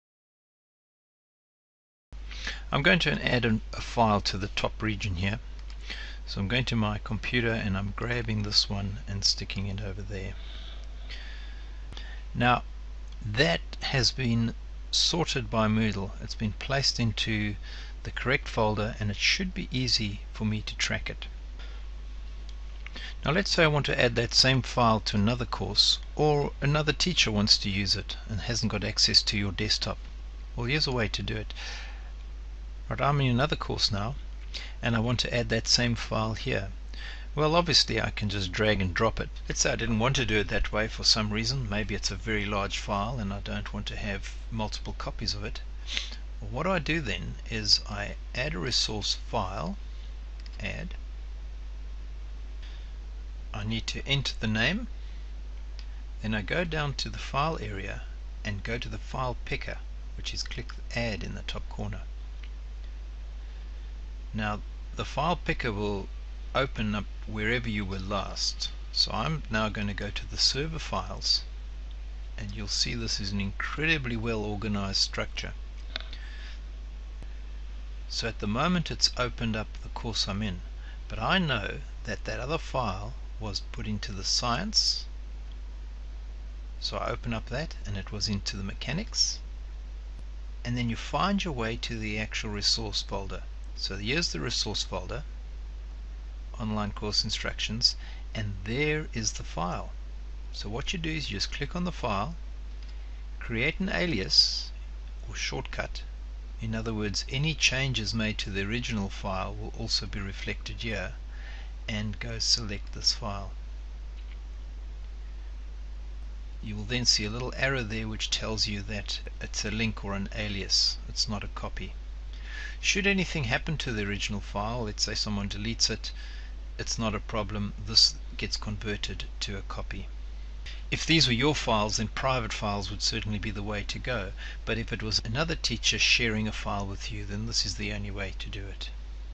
Sharing or reusing files in WeLCom Moodle 2.3
Using Server Files to reuse files across courses.